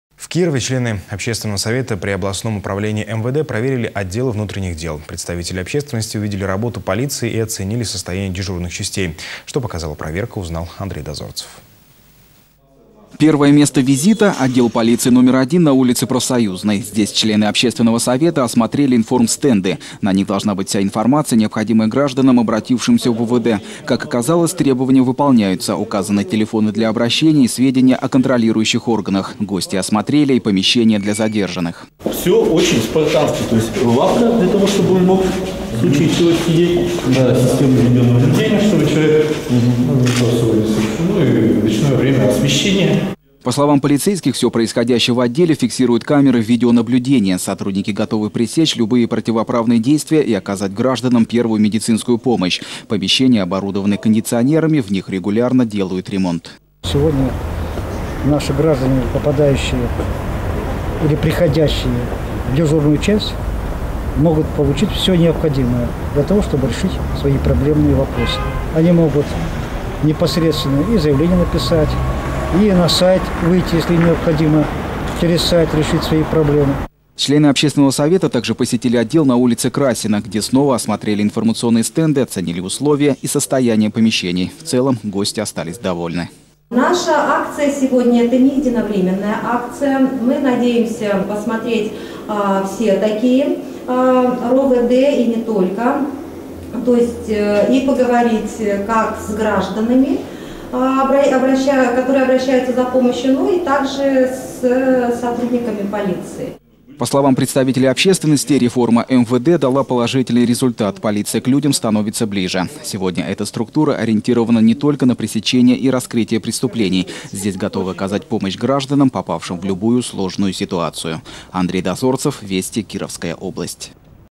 Общественный совет УМВД (ГТРК Вятка)
В Кирове члены Общественного совета при областном Управлении МВД проверили отделы внутренних дел. Представители общественности увидели работу полиции и оценили состояние дежурных частей.